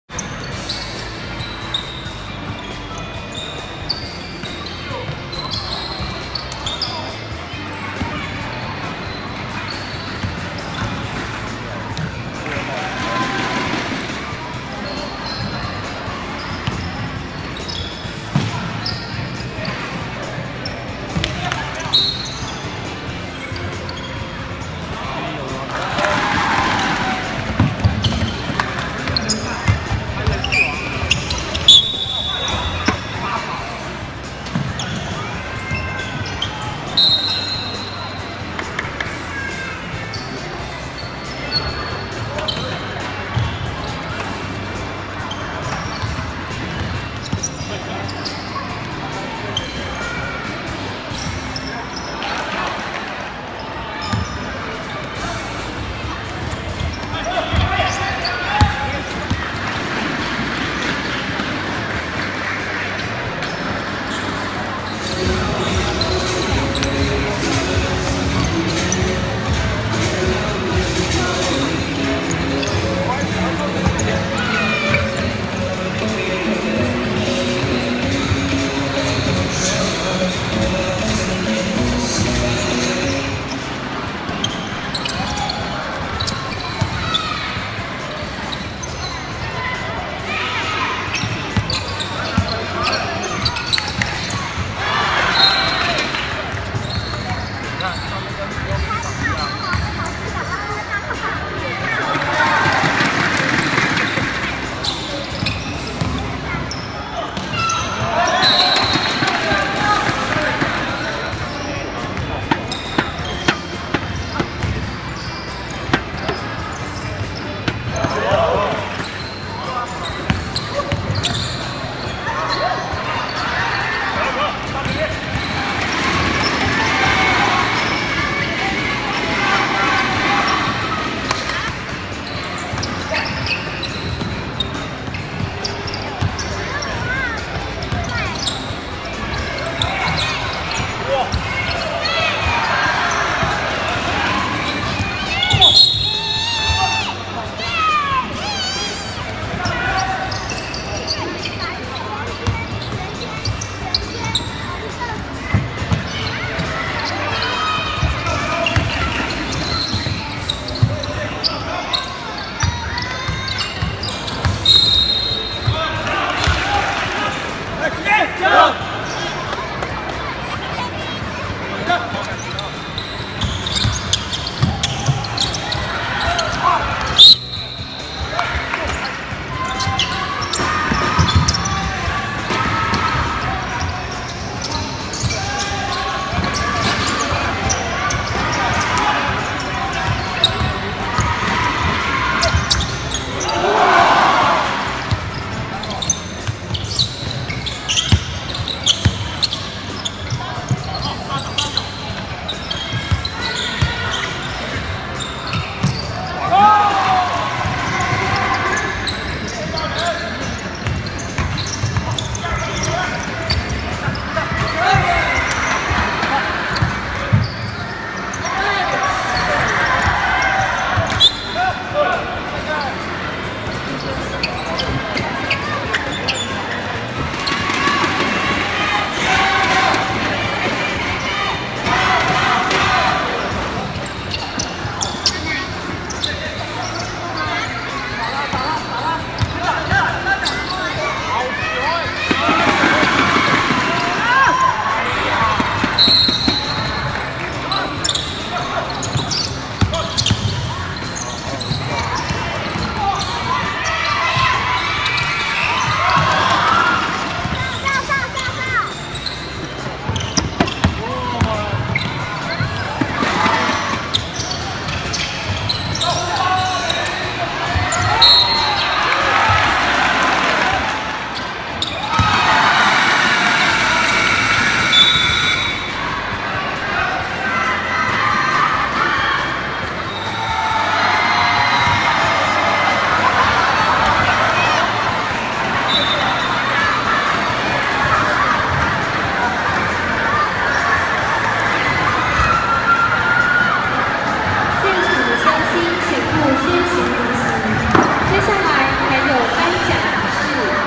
馬祖日報2017/05/26影音／健康上籃26日決戰夜 飛龍險勝玄天奪冠
日期：2017/05/26晚間18:00-晚間22:00
地點：南竿體育館
攝影：曹重偉

　第十四屆健康上籃26日精彩決戰；飛龍免稅迎接玄天家族的強力挑戰，靠著充沛戰力與較高命中率維持優勢，玄天眾將雖奮力一搏，但還是功虧一簣，就由黃鎮、張家榮以穩健表現，帶領球隊在驚險拿下勝利，也是同班底隊伍的五連霸！季軍戰則是衛教聯隊以84比67擊退糾查隊。
　26日（五）戰績：
第31場次：季軍戰－衛教聯隊84比67勝糾查隊。
第32場次：冠軍戰－飛龍免稅93比89勝玄天家族。